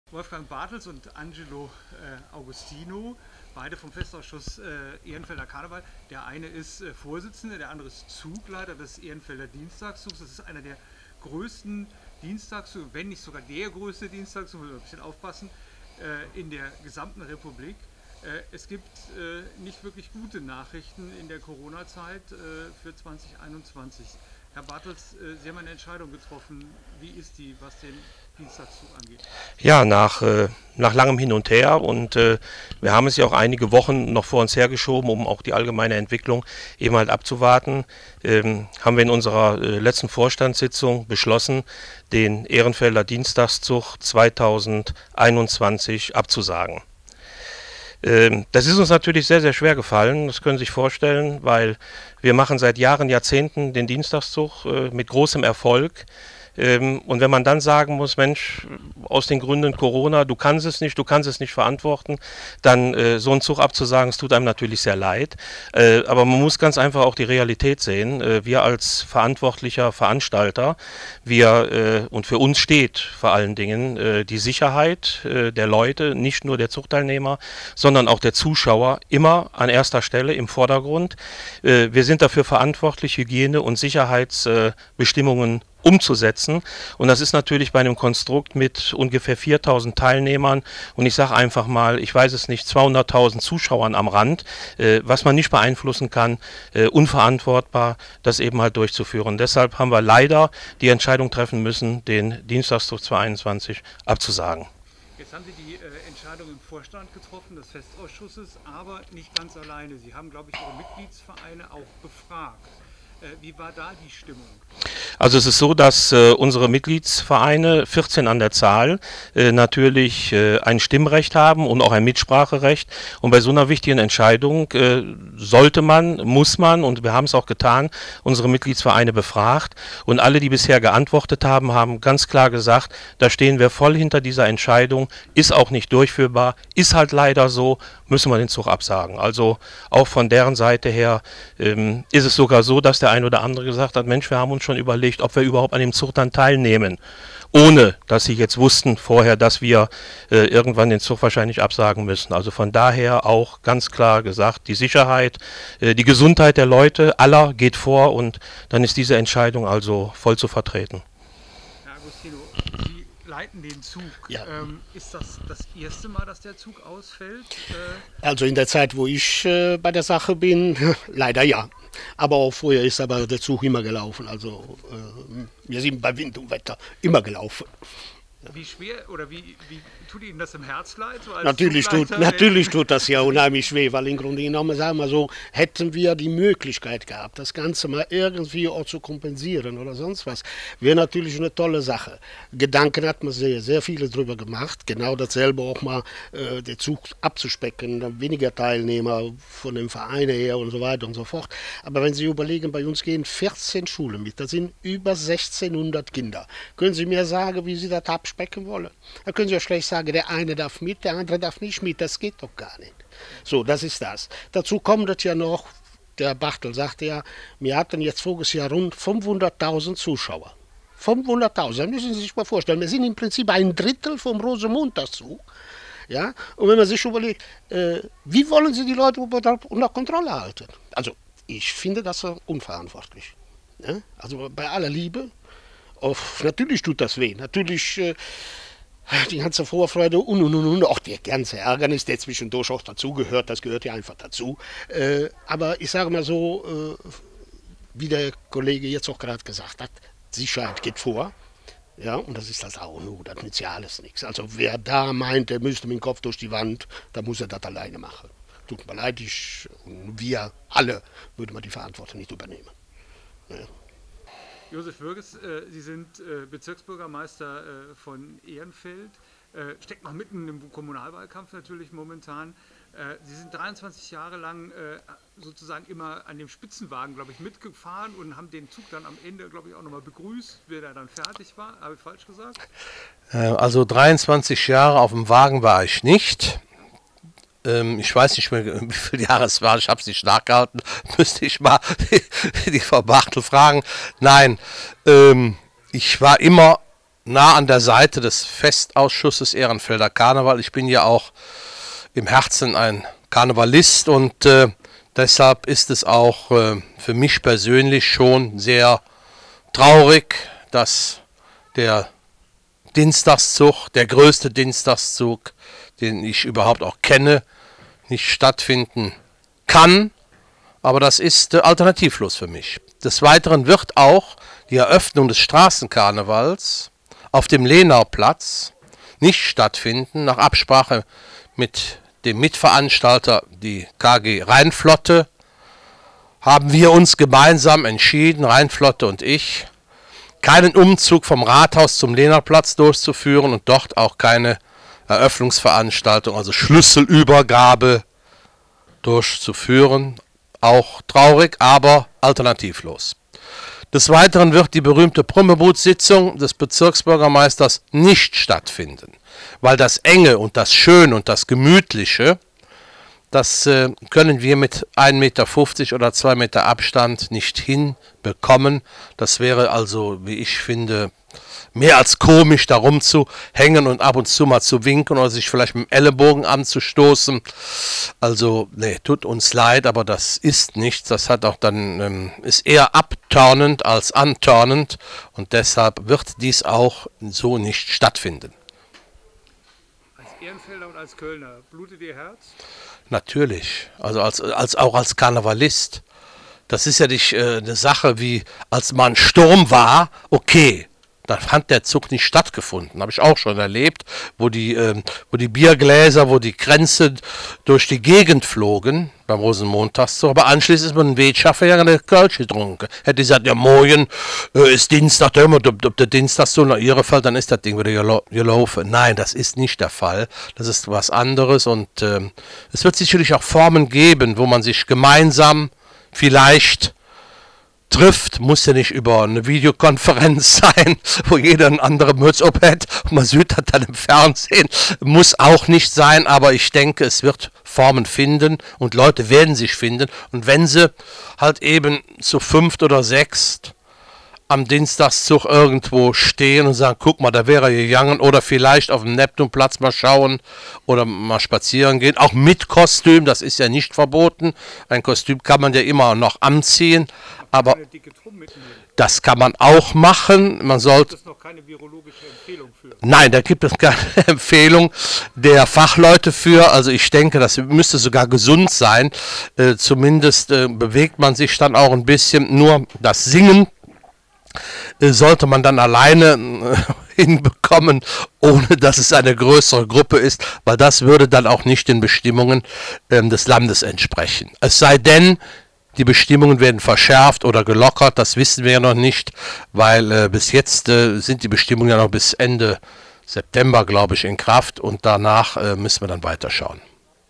Ehrenfelder Dienstagszug 2021 abgesagt
Köln | Der Ehrenfelder Dienstagszug wird in der Session 2021 nicht stattfinden. Das hat der Festausschuss Ehrenfelder Karneval entschieden und auch seine Mitgliedsvereine befragt. Der Grund: Die Corona-Zeit. Die Verantwortlichen schaffen Planungssicherheit für ihre Teilnehmer. Auch Bezirksbürgermeister Josef Wirges sagt Veranstaltungen ab. Bringen wir es auf den Punkt: Die Ehrenfelder Jecken sind traurig, handeln aber verantwortungsbewusst.